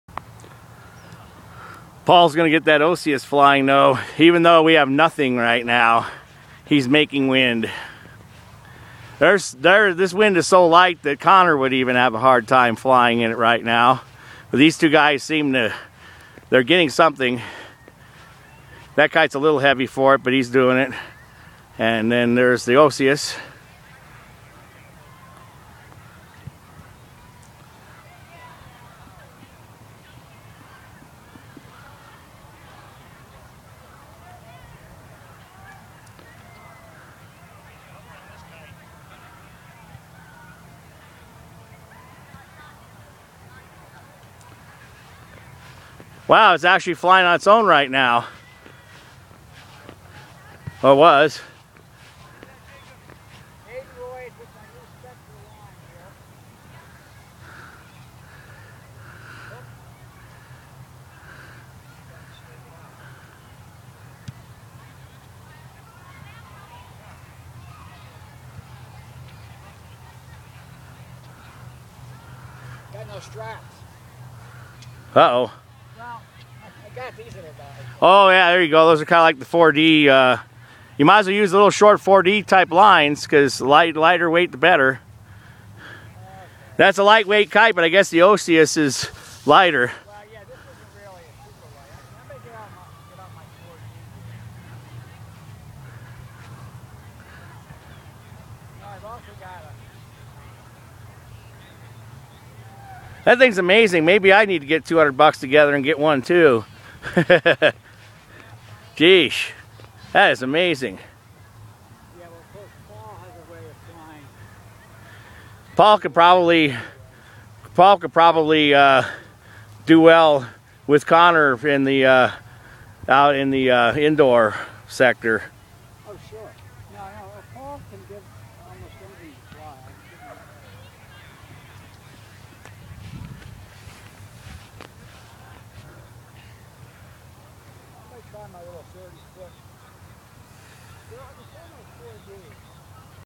Very light wind at Clement May 22nd 2011 fly
Sky Burner's OCIUS kite, the ANSWER for Pathetic Colorado
Paul Glasspoole Pilot